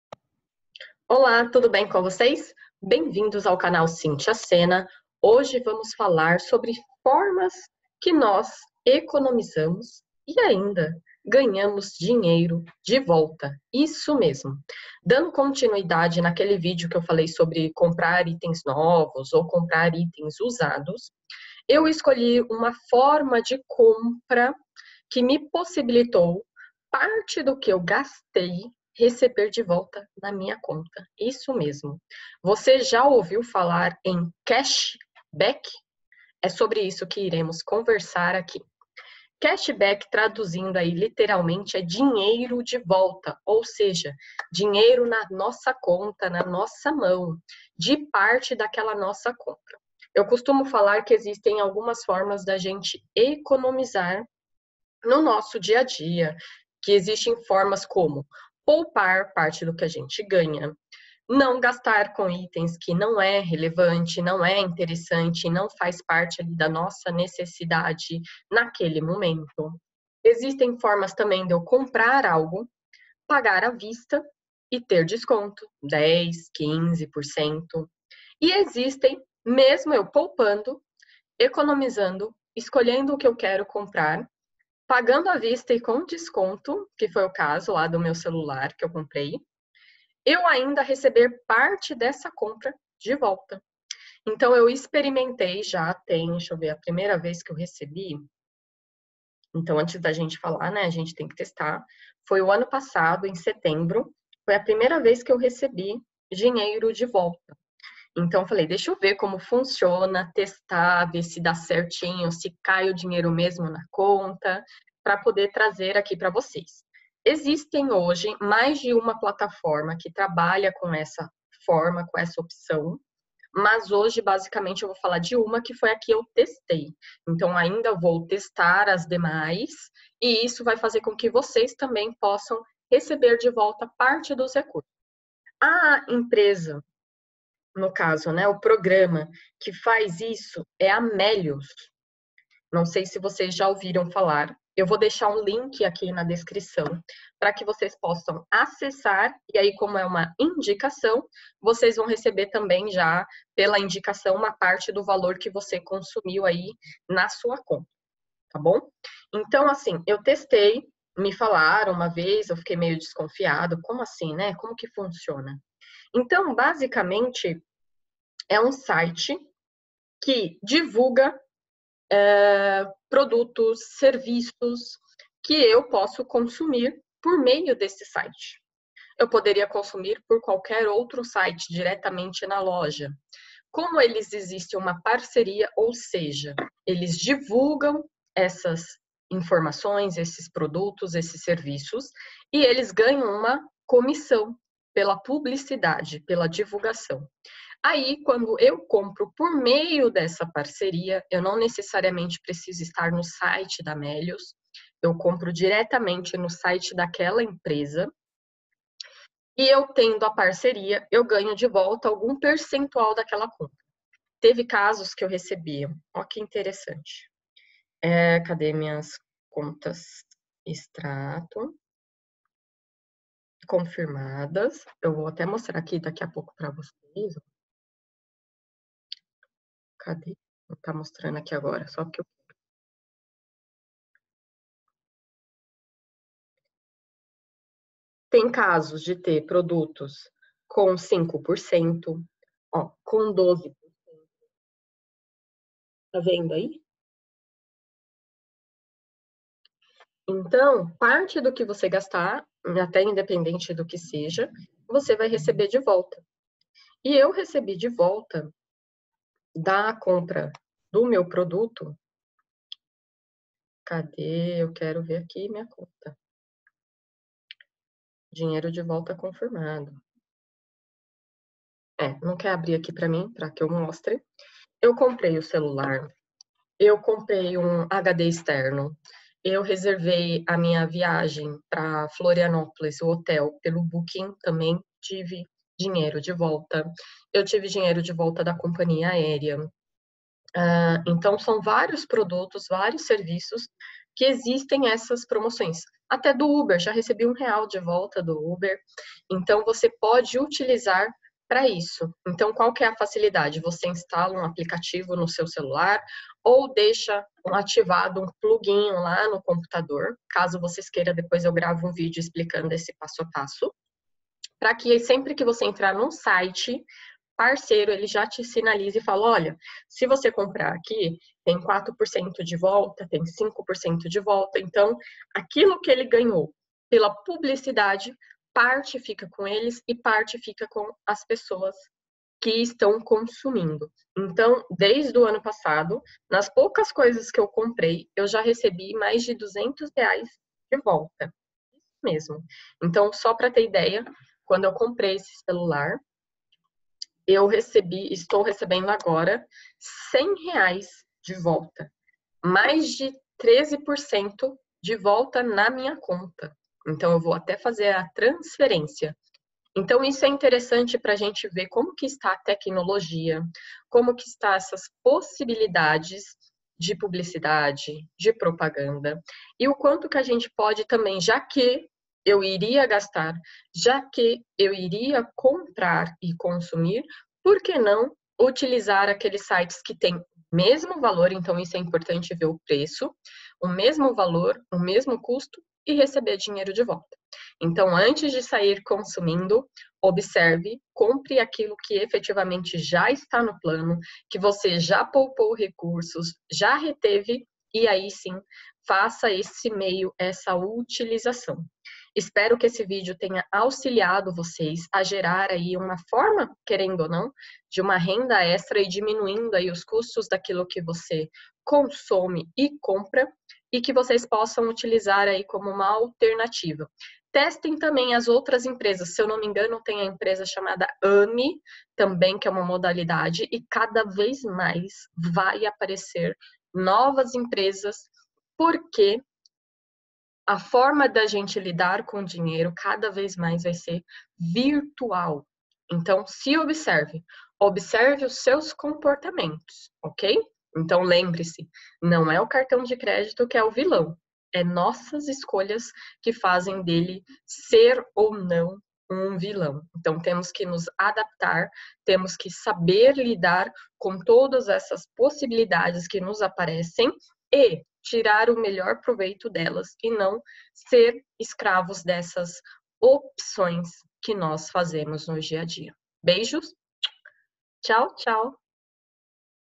Ganhe Dinheiro de Volta | Uma forma de economizar
Olá,
Você já ouviu falar em cash back?
Já pensou em receber de volta parte do que gastou na aquisição de produtos e serviços?
Hoje em dia já existe essa possibilidade.
Assista ao vídeo e veja como reverter parte do seu consumo como economia e ter assim dinheiro na sua conta.
Pesquise, compare, poupe, economize e ganhe de volta!

Espero que seja útil a vocês, e se tiverem mais sugestões de outras opções, me avise para que eu teste e traga a informação aqui para vocês.

Abraços,